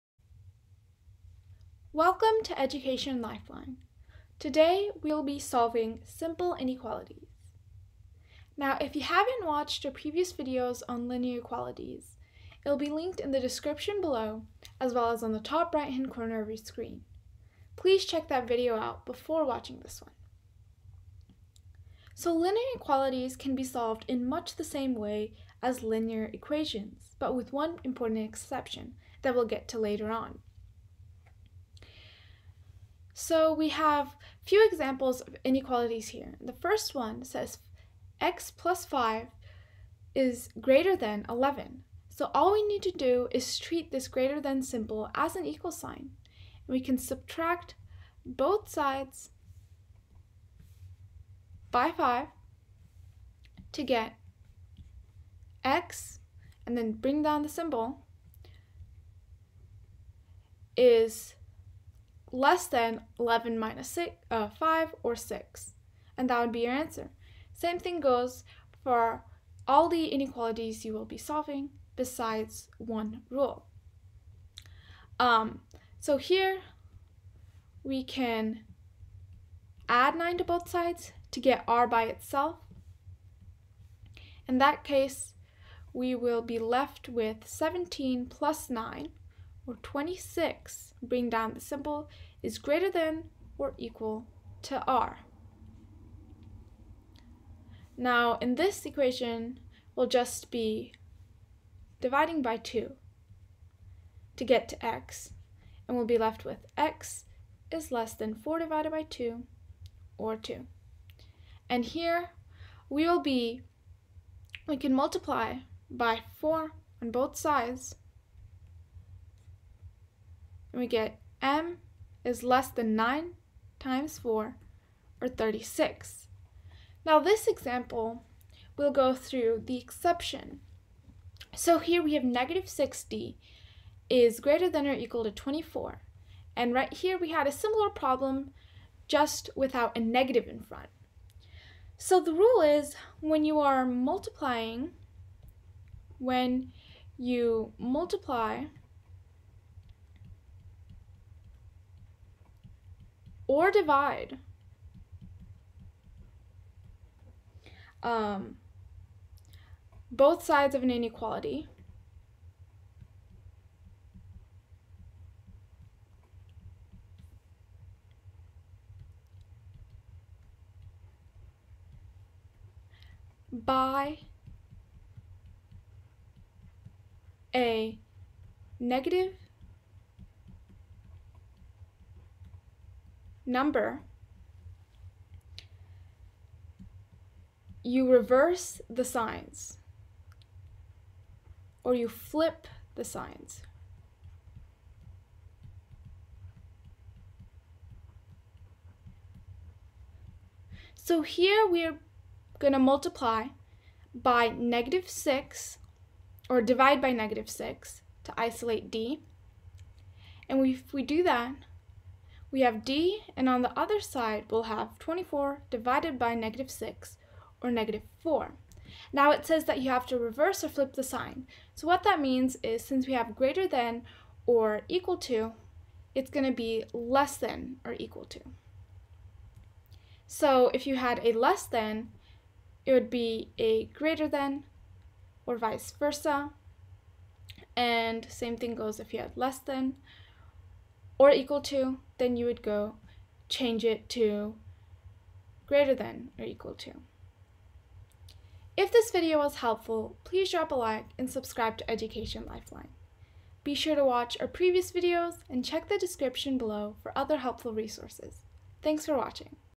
Solving Simple Inequalities - Algebra 1 Unit 5 Lesson 2
In this video, we will be solving simple inequalities or one-step inequalities and explain the exception to solving inequalities.